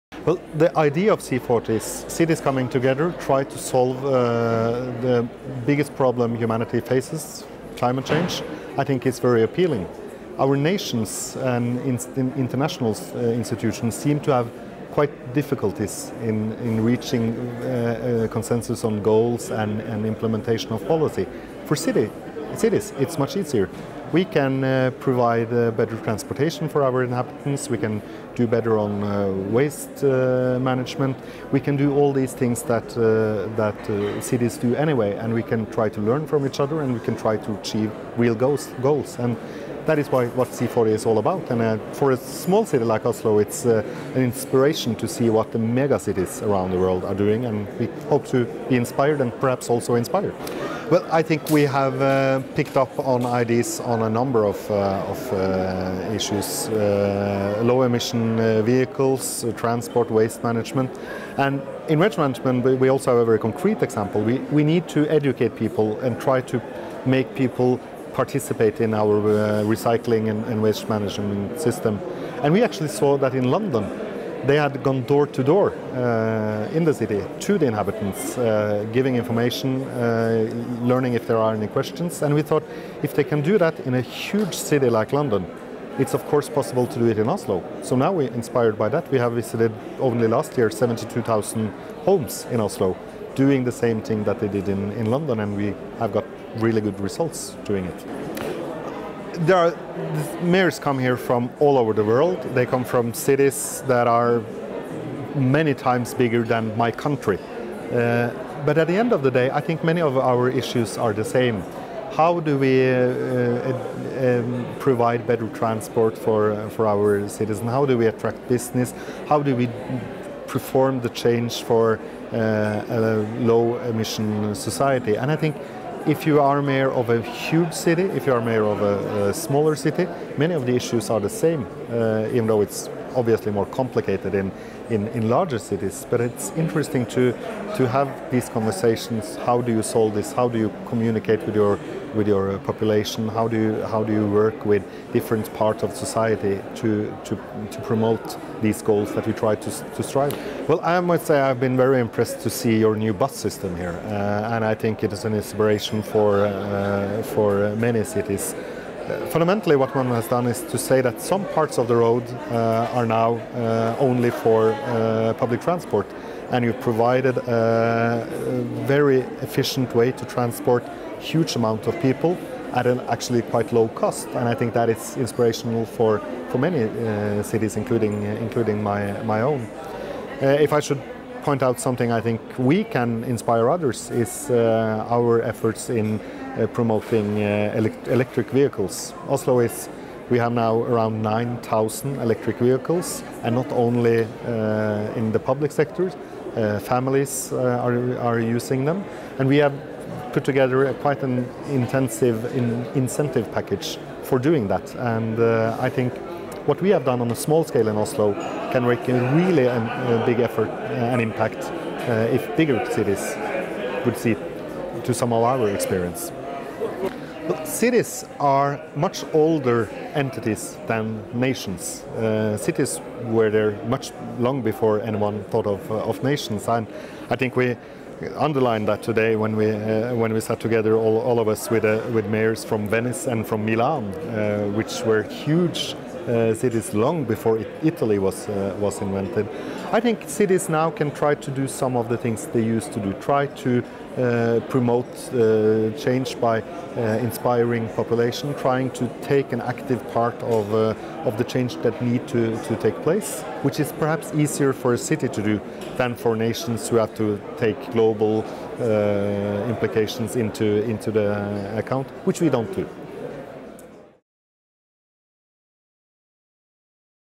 C40 Summit Video Blog Series: Stian Berger Røsland, Governing Mayor of Oslo
In partnership with the City of Johannesburg we conducted a series of video interviews with mayors, leading sustainability officials and partner thought leaders during the C40 Mayors Summit in Johannesburg, South Africa on February 4-6, 2014.